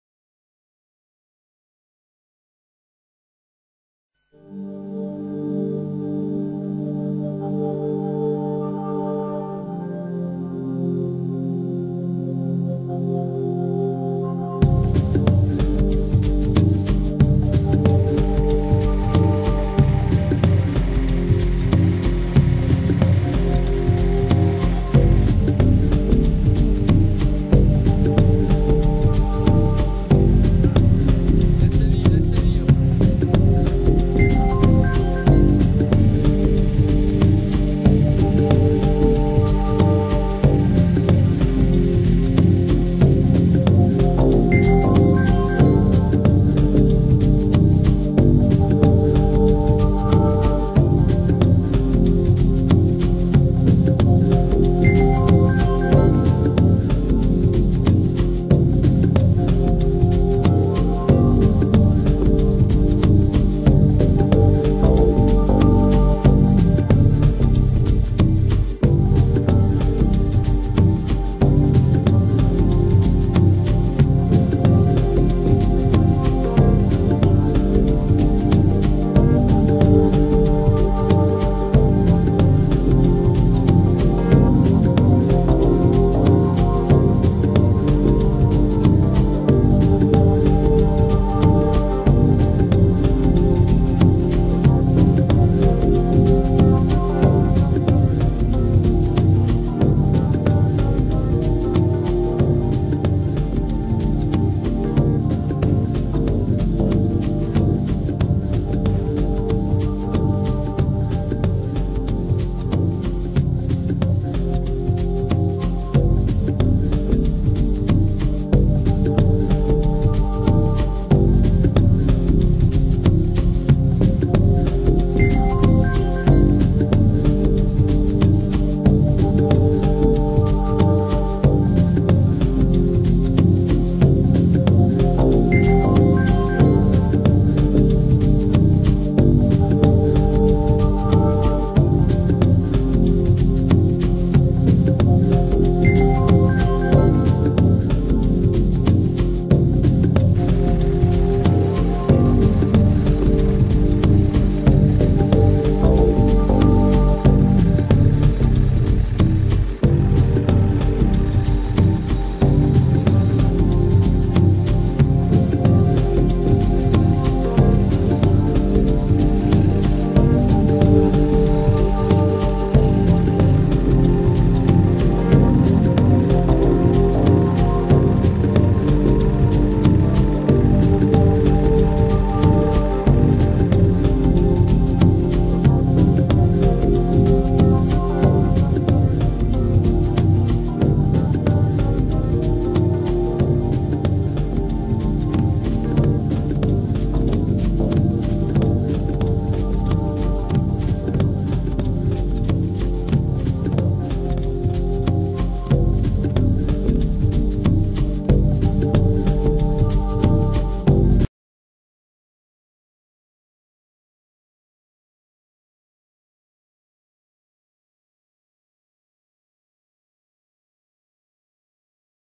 Selladoras de juntas y fisuras Schäfer LS
En este video podemos ver distintos modelos de selladoras de juntas y fisuras marca Schäfer trabajando